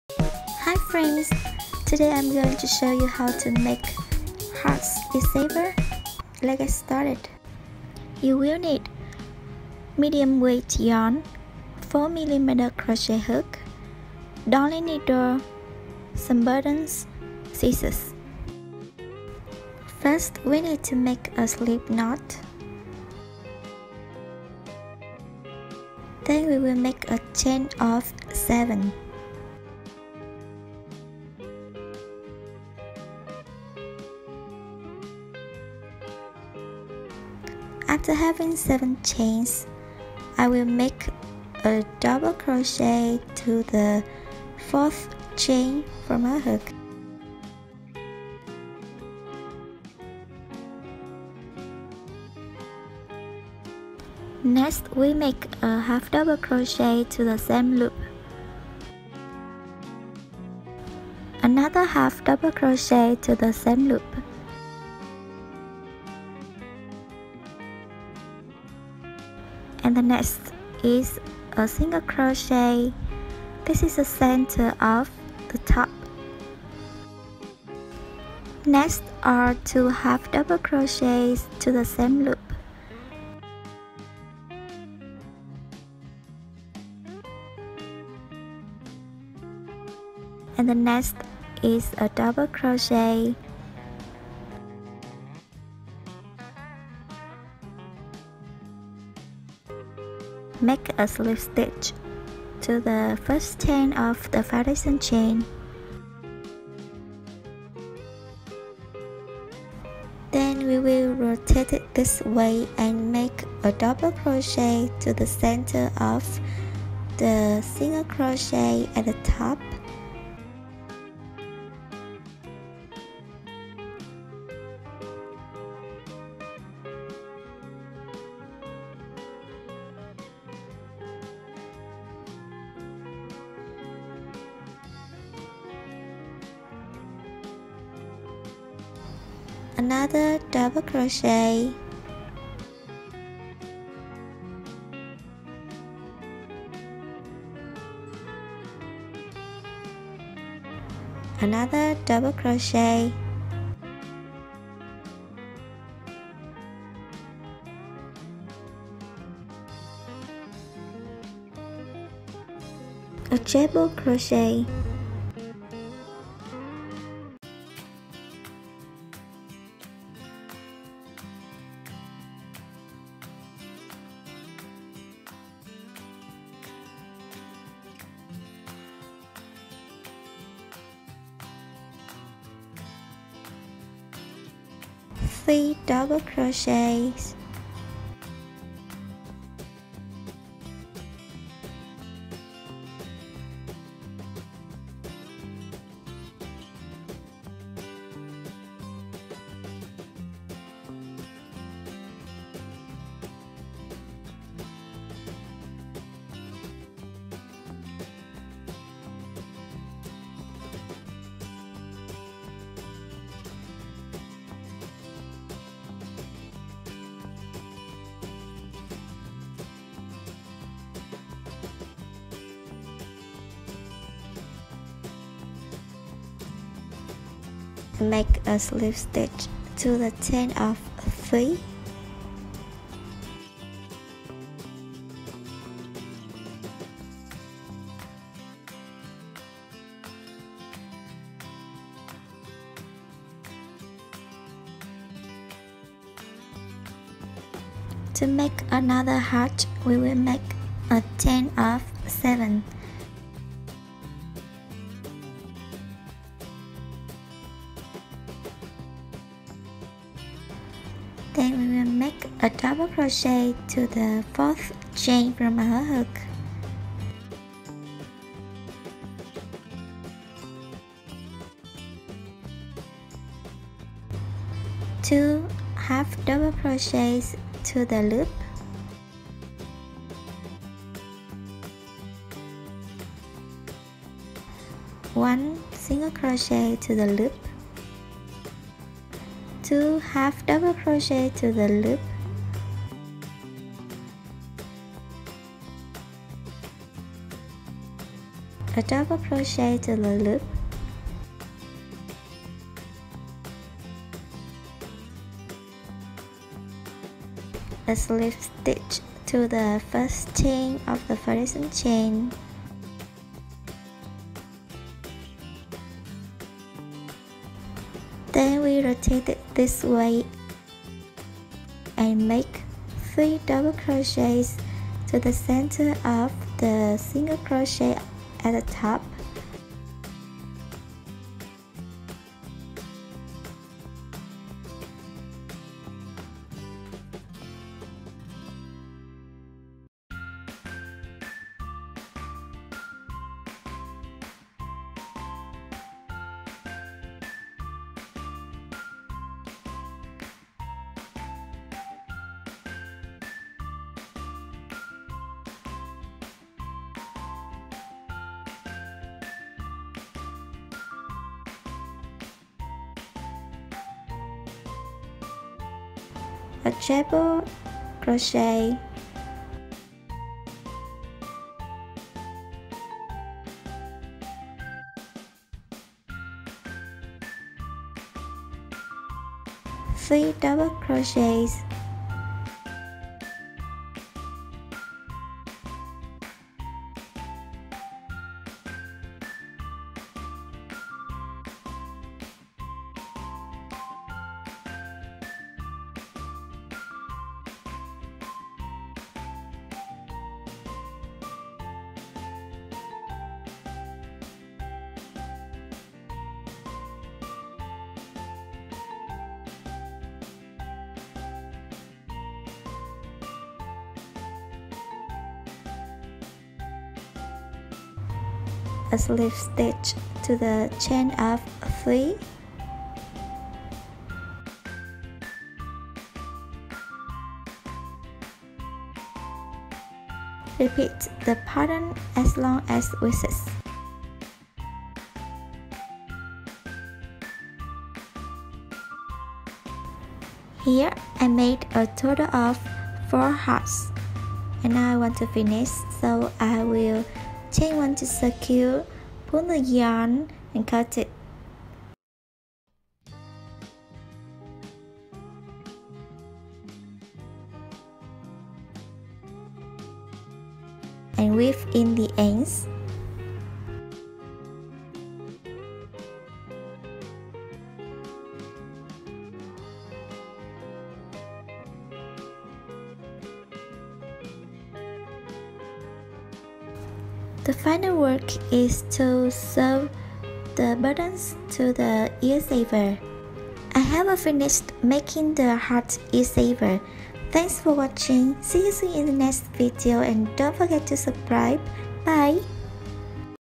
CROCHET EAR SAVER | How to Crochet Heart Ear Saver/Adapter/Extender/Connector/Helper for Face Mask
In this video I will show you how to make an Ear Saver/Adapter/Extender/Connecter/Helper with Heart Chain.

Yarn: Medium Weight
Hook: 4.0 mm